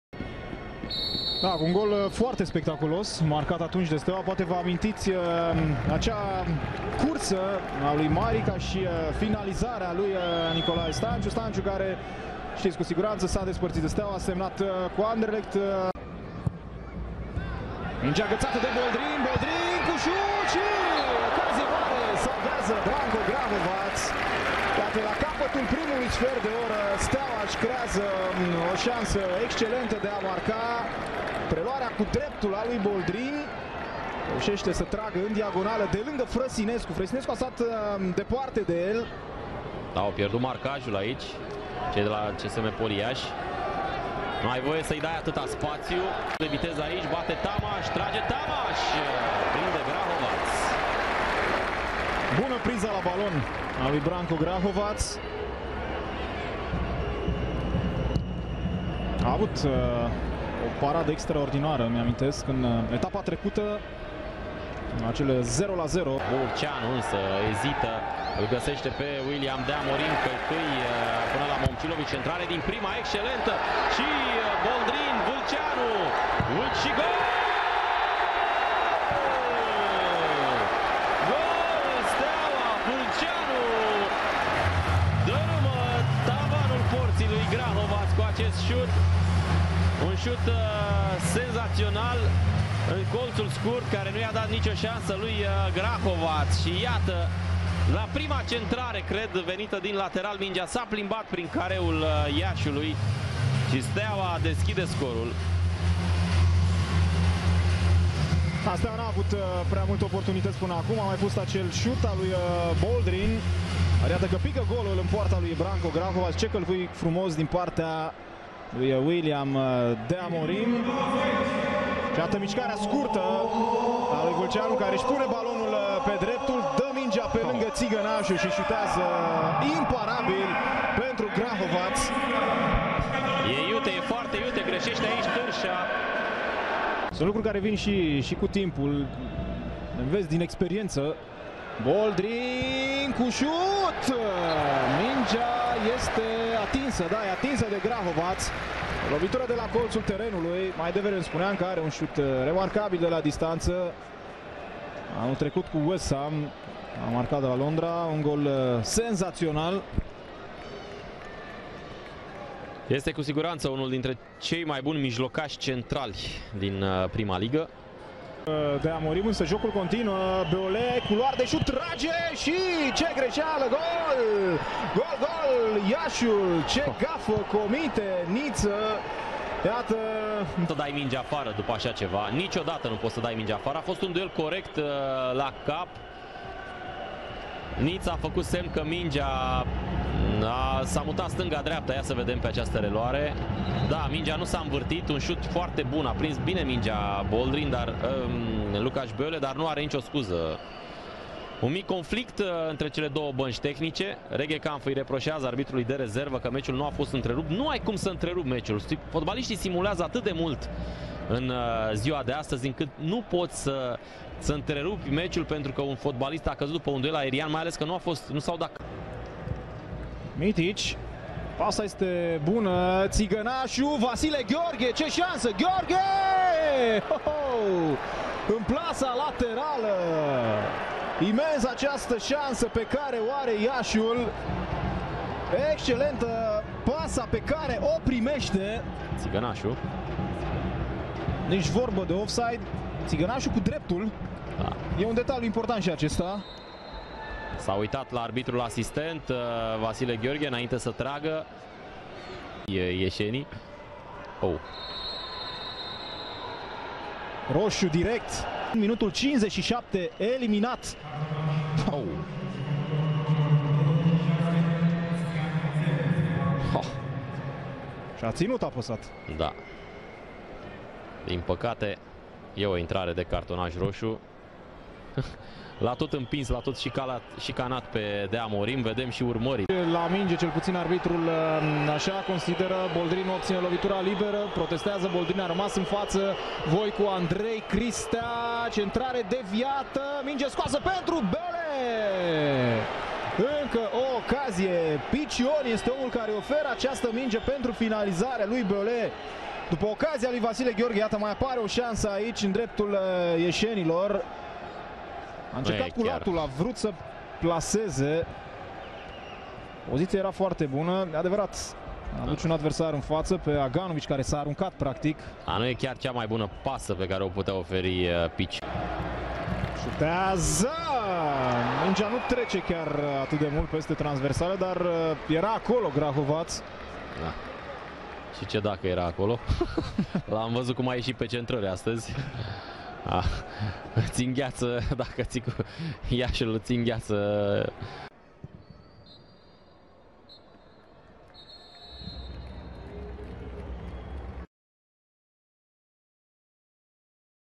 Steaua - CSM Poli Iași 1-1 (31.august.2016)
Robert Vîlceanu (20')/ Lukacs Bole (42')